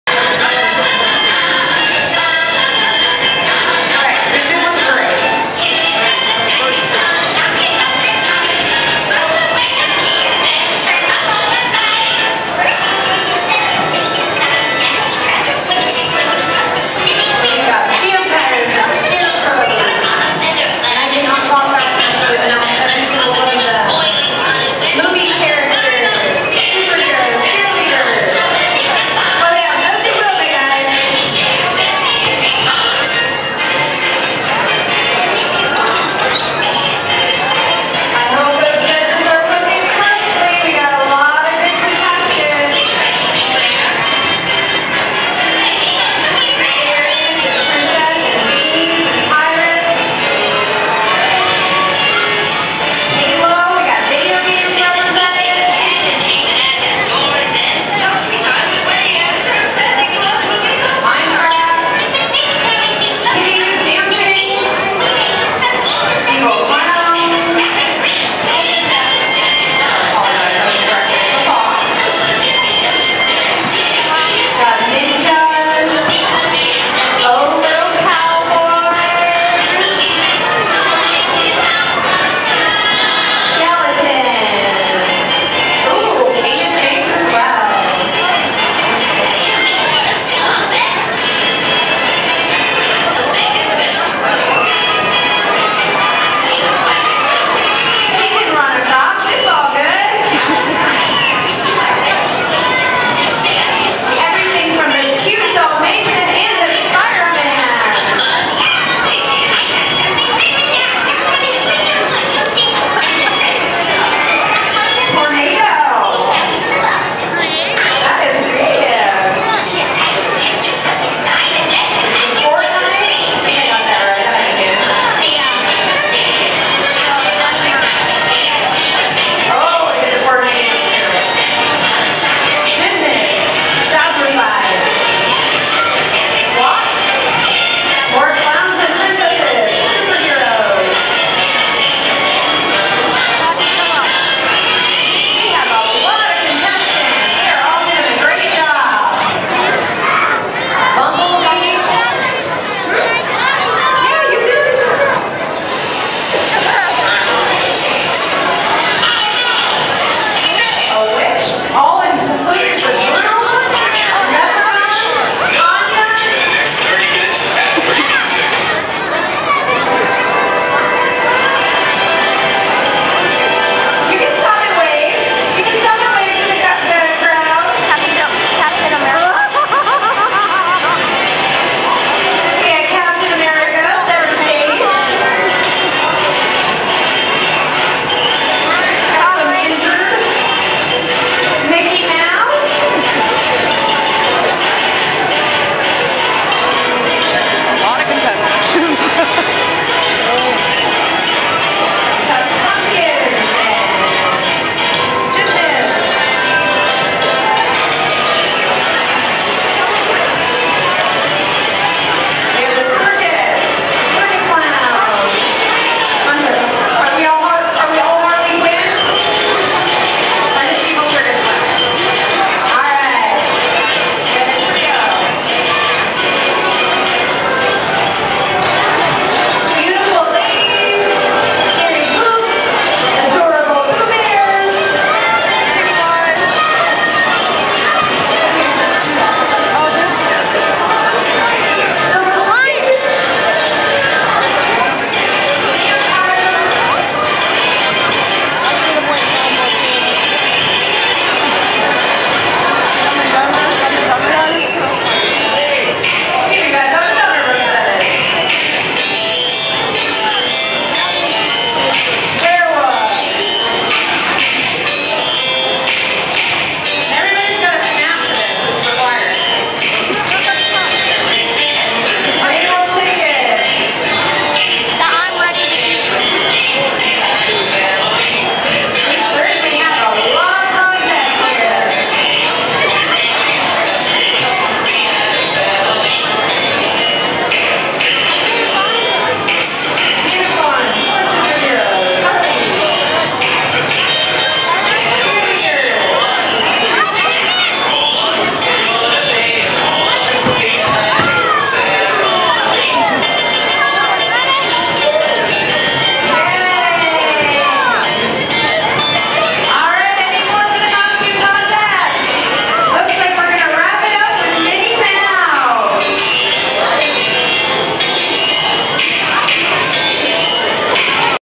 parading costumes
Shawnee expo's Halloween Town had a show of hundreds of residents Monday night during its inaugural event.